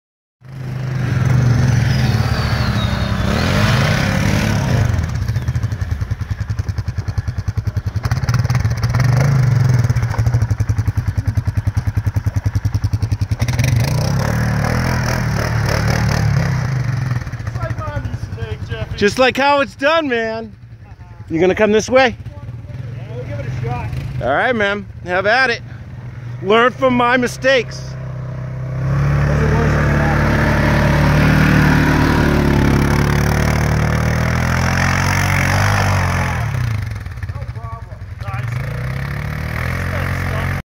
ATV trail riding Dunbarton New Hampshire 12-26-17 Learn from my Mistakes
On the trails today in Dunbarton New Hampshire and had some fallen trees along the way. Didn't have any tools to move the tree's so we went around. There were atv's here before us and they made a path around and I got stuck but was able to get around. Took some ribbing about it but that's all part of the ride.